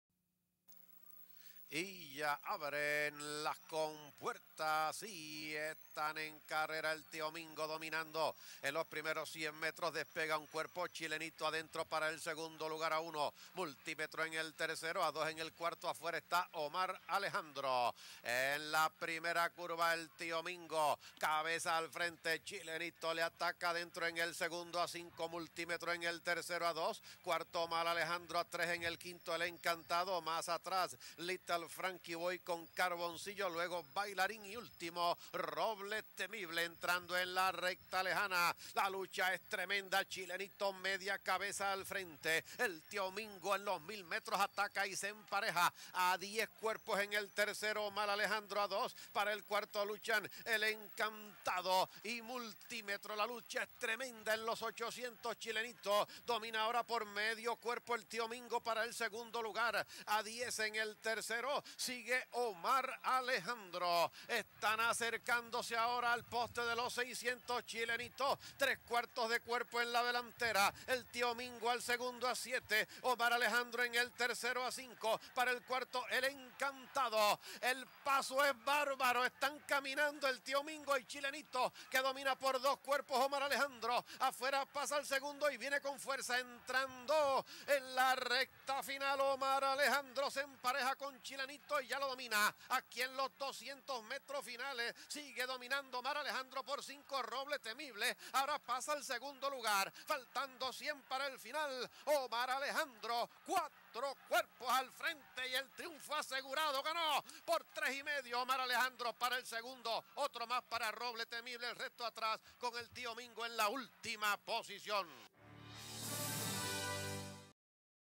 OMAR ALEJANDRO - Derby Puertorriqueño 2004
El nativo Omar Alejandro, montado por Joel Cruz, se impone en el Derby Puertorriqueño celebrado en el hipódromo El Comandante, el 2 de mayo de 2004. Era propiedad del establo Fanucci Racing y entrenado por Manuel Fernández. Cronometró 1:45 4/5 para 1,700 metros.